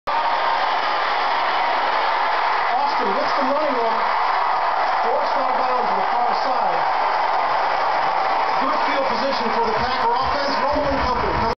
Bryant Gumble Bumble
How the mighty have fallen!  Bryant Gumble, play by play dinosaur for the NFL Network flubs a call by calling "Good field position for the Packer offense, Romo and company..." after Dallas ran back a kick-off to mid field.